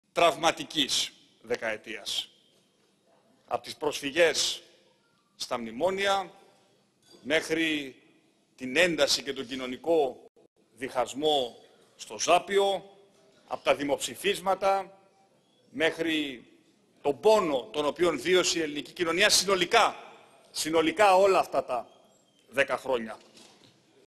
Το "άδειασμα" στον Σαμαρά που... μαζεύτηκε στην απομαγνητοφώνηση.
Δείτε το σχετικό απόσπασμα από τη χθεσινή ομιλία του πρωθυπουργού στη Βουλή.